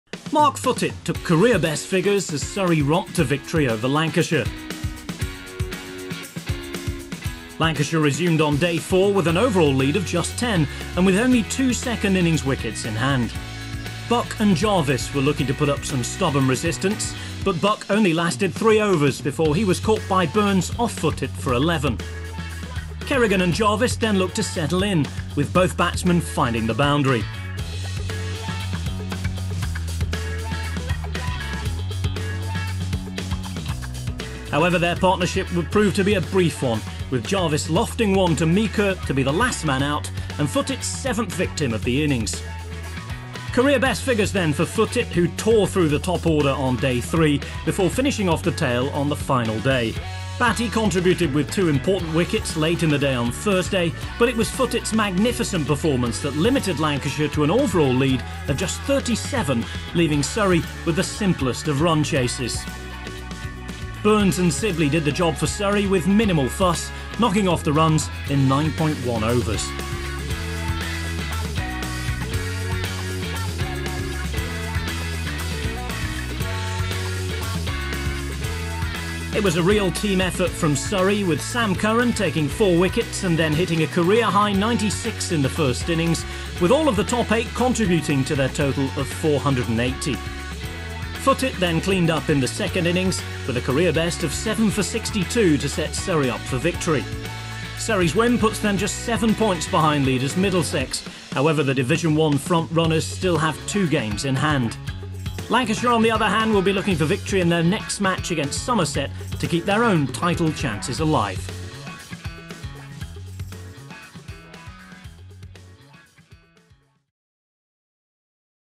Footitt fires Surrey to 10-wicket win - Surrey v Lancs, Day Four
Ten months into his Surrey career and having suffered early-season niggles and lapses in confidence, left-arm seamer Mark Footitt finally announced his arrival at the Kia Oval with a career-best haul of seven for 62 to wrap up Surrey’s 10-wicket win over Lancashire.